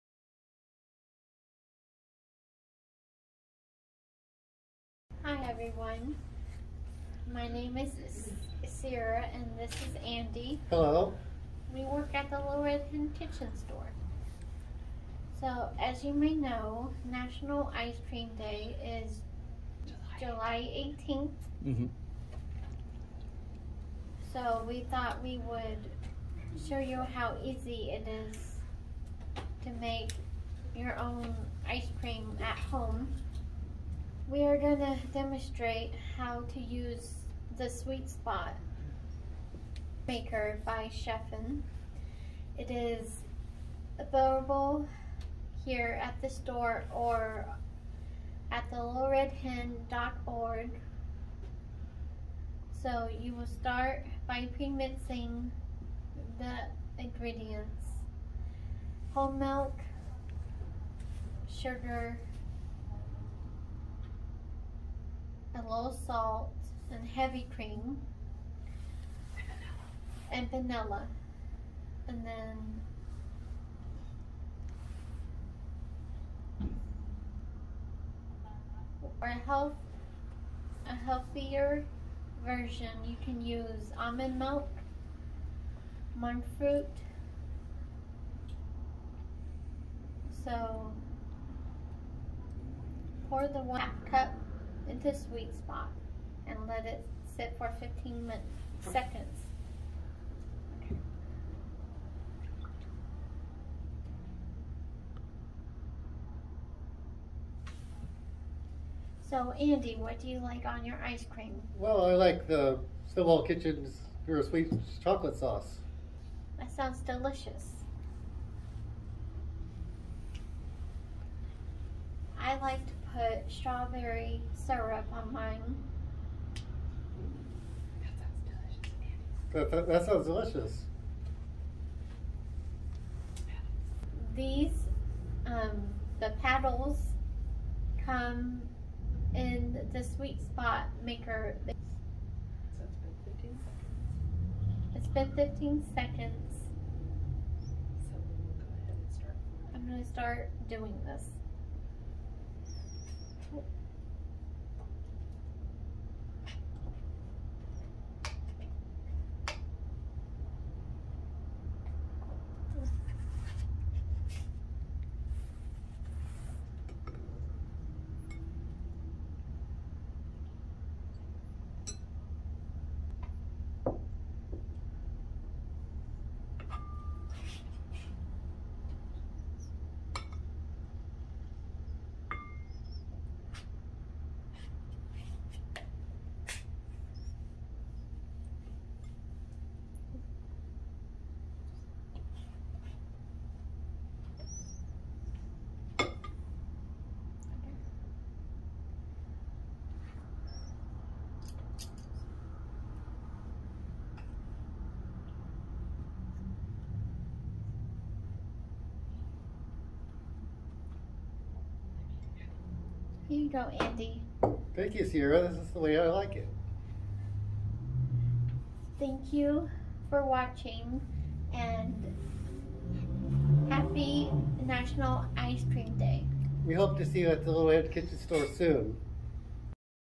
Home Made Icream at The Kitchen Store for National Ice Cream Day!
Sierra and Andy show us how easy it is to make your own homemade ice cream with the Chef'N Sweet Spot Ice Cream Maker. Available at Little Red Hen Kitchen Store or online at Littleredhen.org.